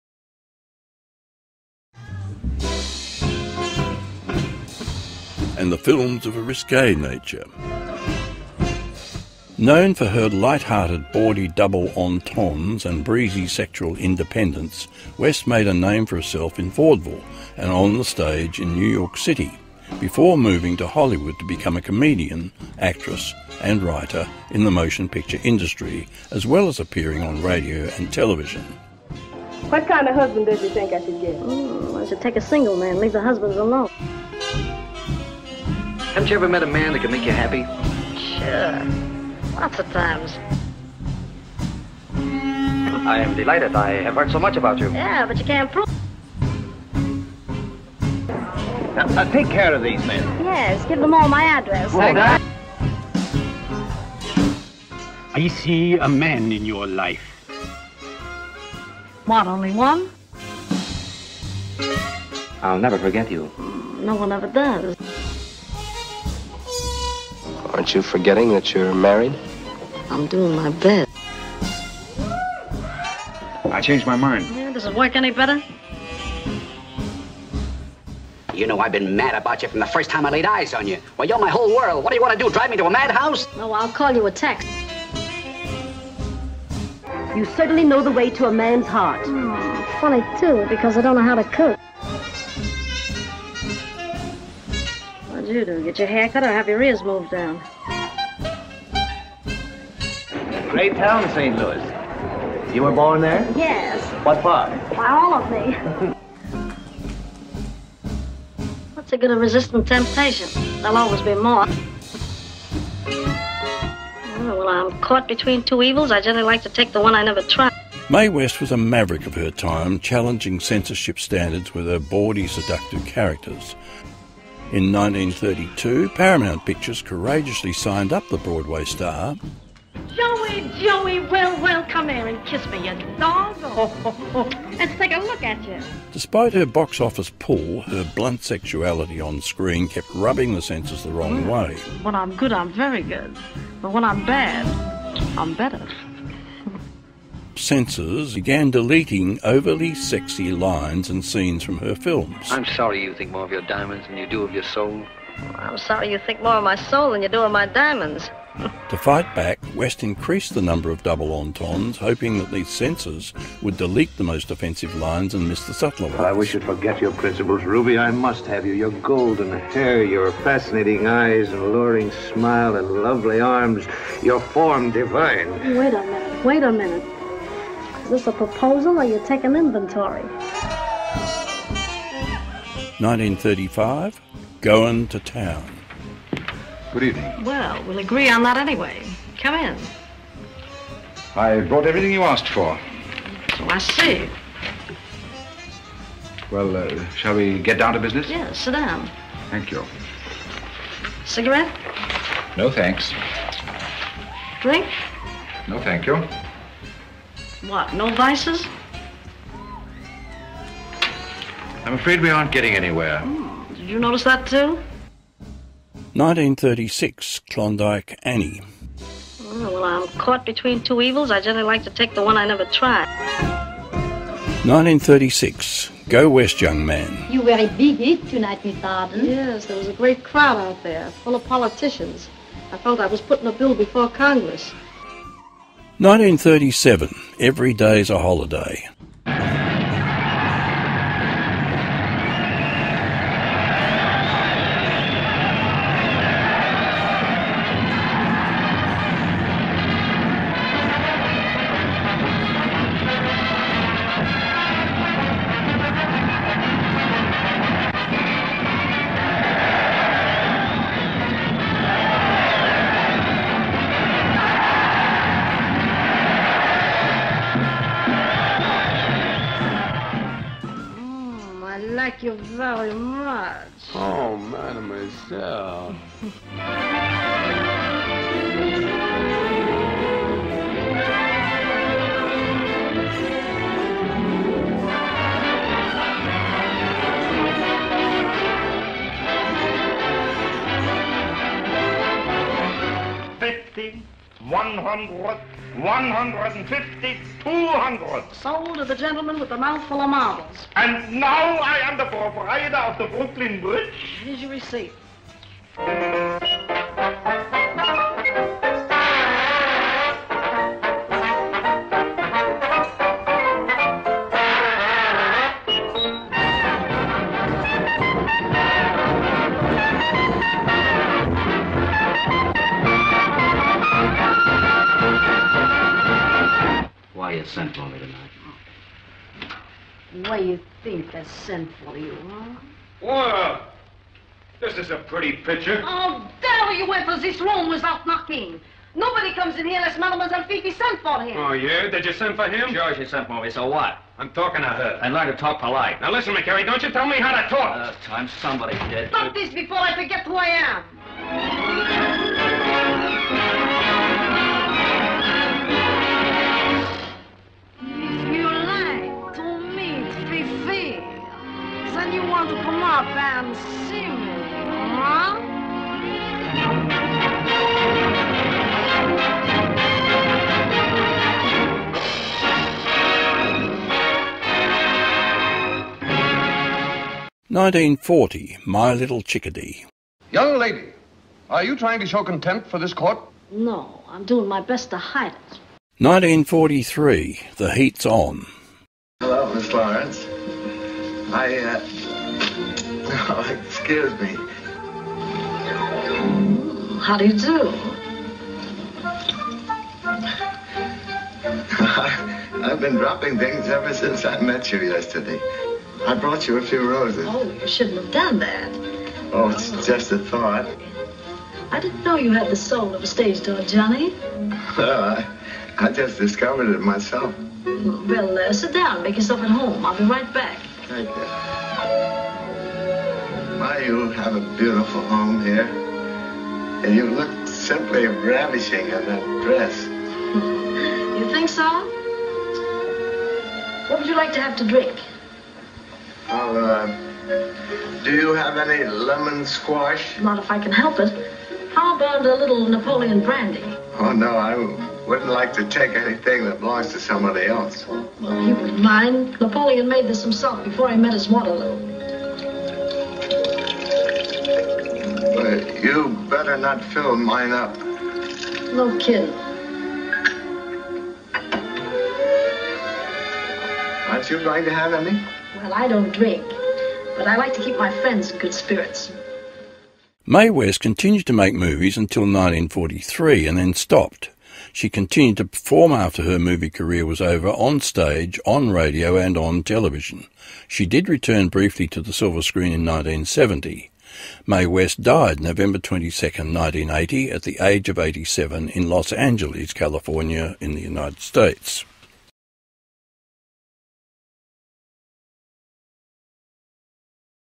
Mae West (1893–1980)
Mae West was an American actress, singer, playwright, screenwriter, comedian, and sex symbol whose entertainment career spanned seven decades.

Born in a working-class section of Brooklyn, she first entered the world of "show business" at the age of five entertaining church locals. She began performing professionally in vaudeville at the age of fourteen. Eventually she moved on to Broadway. She encountered many censorship problems during her career and was even prosecuted on morals charges in 1927.

Known for her lighthearted bawdy double entendres, and breezy sexual independence, West made a name for herself in vaudeville and on the stage in New York City before moving to Hollywood to become a comedian, actress, and writer in the motion picture industry, as well as appearing on radio and television.

She became a show-business giant, a personality so distinctive she transformed forever the way women and sex would be presented on stage, in films, radio, TV and cabarets around the world.

For her contributions to American cinema, the American Film Institute named West 15th among the greatest female stars of classic American cinema.

Mae West was a maverick of her time challenging censorship standards with her bawdy, seductive characters. Throughout the 1930s, she appeared in such films as I'm No Angel, She Done Him Wrong and Klondike Annie.

At 33 years old, she wrote, produced, directed and starred in her first Broadway play entitled "Sex," a 1926 production deemed so obscene that she landed in jail for it. She was the queen of double entendres on and off screen, delivering some of the best-remembered quips in movie history.

In 1932, Paramount Pictures courageously signed up the Broadway star. It was quite a feat at the time, for she was already considered a "mature" actress, but West looked younger than her 38 years

Despite her box office pull, her blunt sexuality onscreen kept rubbing the censors the wrong way. In 1934, censors - in care of the infamous Hayes Code of onscreen ethics - began deleting overtly sexy lines and scenes from her films. To fight back, West increased the number of double entendres, hoping that the censors would delete the most offensive lines and miss the subtler ones.